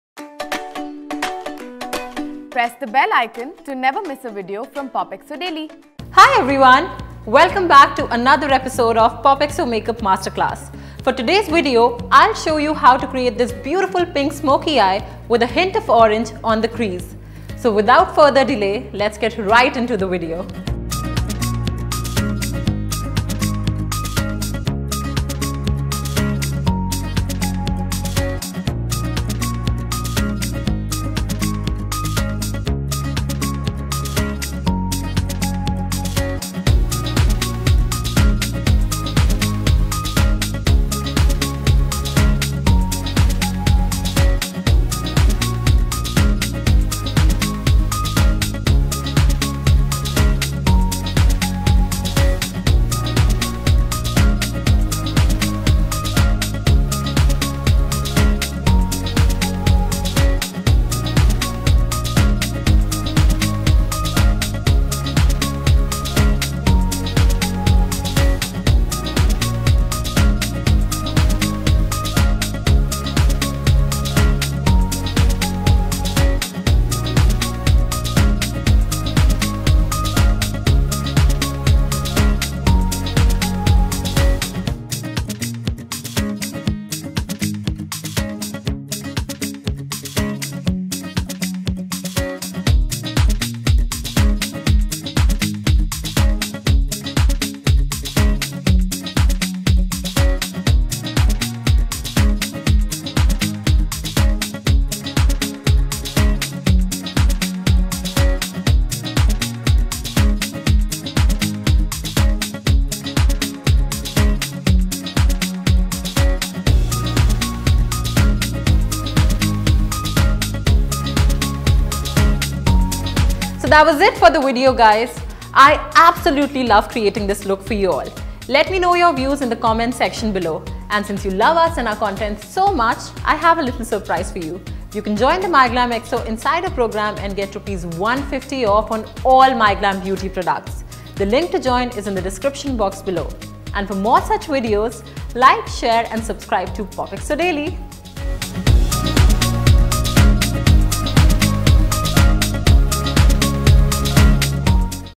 TEQUILA SUNRISE MAKEUP TUTORIAL | By Siddhi Gupta | POPxo Makeup Masterclass
Spring is all about bright shades & joyous mood. Bringing together all the springy elements, we created a pink smokey eye look with a dash of orange in today's POPxoMakeupMasterclass. Check it out!